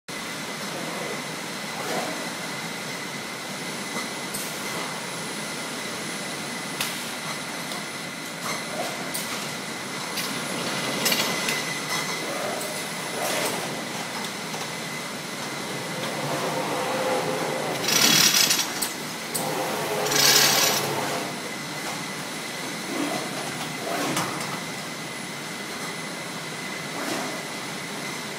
High speed product sorter and pallet packer - how it works
Pack product enters and begins a game of Tetris where the packs are orientated based on pack size to fill a pallet layer.
Once the pallet layer is complete the sweeper sweeps it onto the blind which is closed.
The blind rotates and lowers to the next pallet index position and opens to deposit the pallet layer.